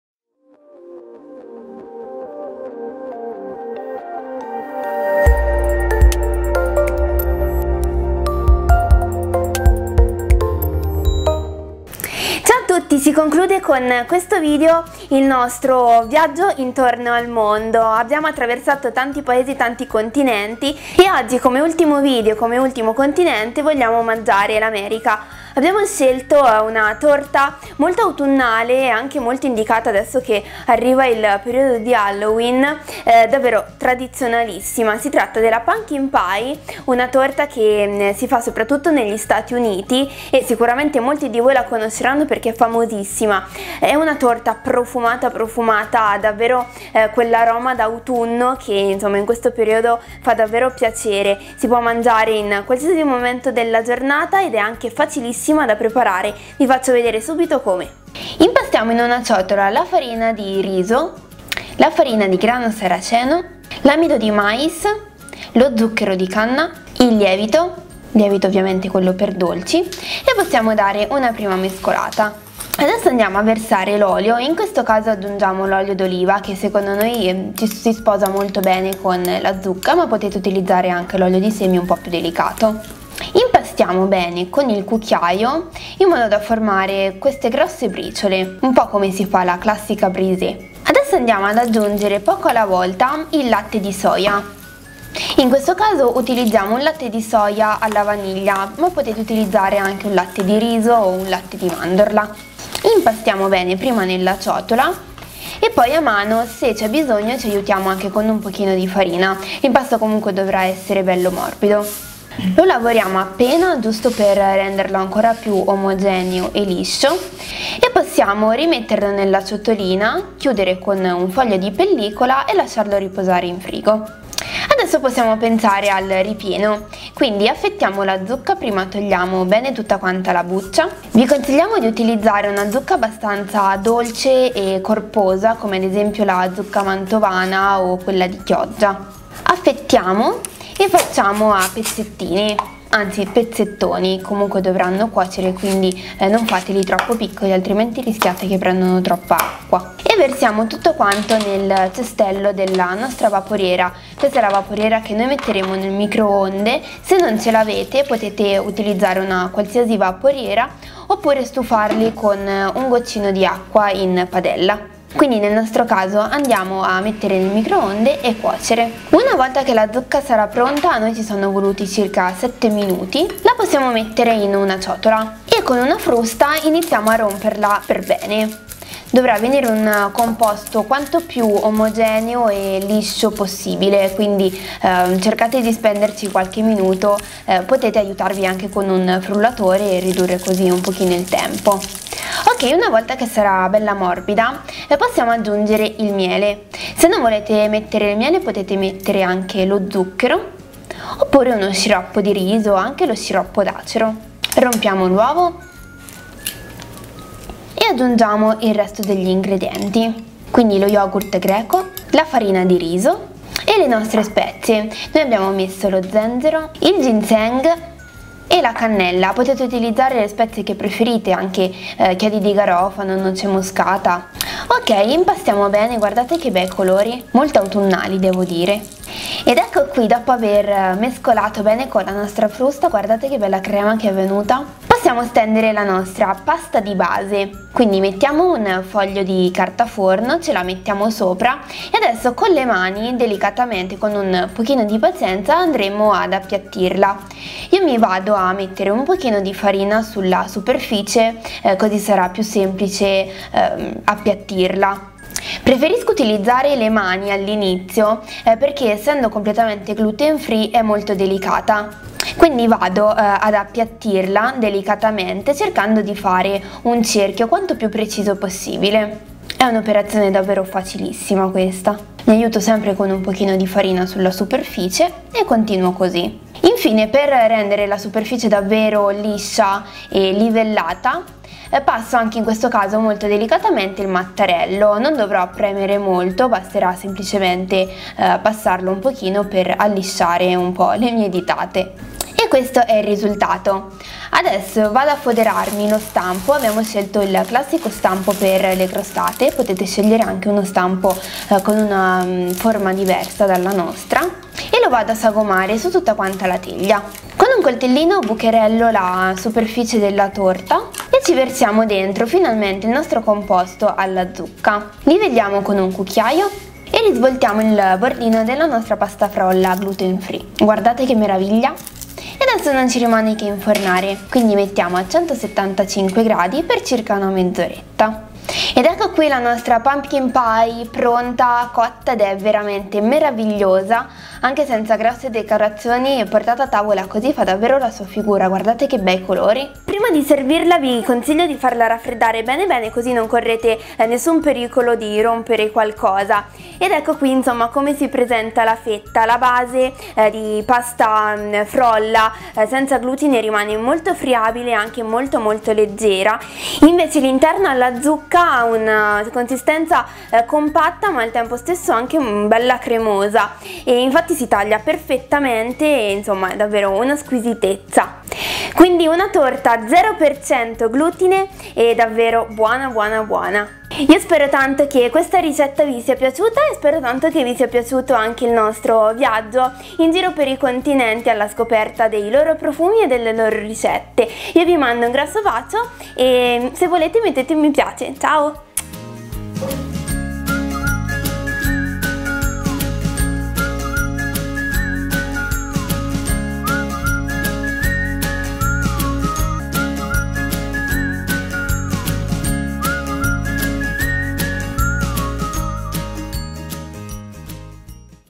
Ricetta Pumpkin Pie - Beauty Club Expo 2015 edition
In occasione di EXPO 2015 Deborah Milano in collaborazione con le bloggers de Il Goloso Mangiar Sano vi offre 5 ricette semplici, sane e sfiziose. Ogni ricetta vi condurrà alla scoperta delle abitudini culinarie di un paese diverso, senza dimenticare mai che la prevenzione inizia da una alimentazione naturale, sana e controllata.
Deborah Milano sostiene la ricerca, sostiene Pink is Good, fondazione Umberto Veronesi

La ricetta del mese di ottobre è la Pumpkin Pie, la classica torta di zucca della tradizione americana, ideale con l'autunno.